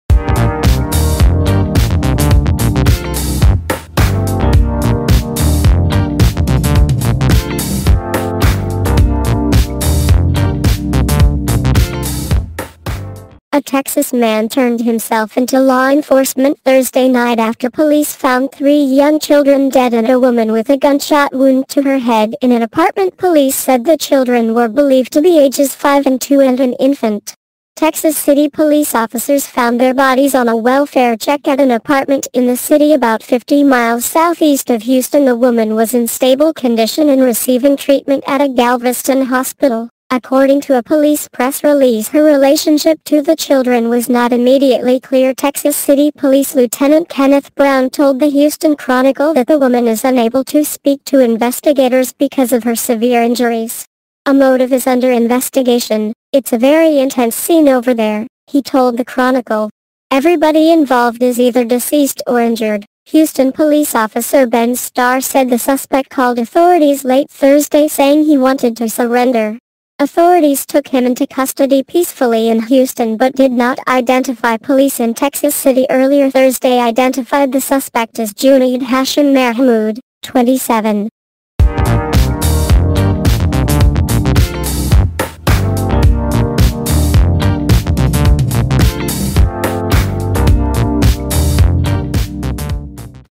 3 children killed in Texas, suspect in custody
3 children killed in Texas, suspect in custody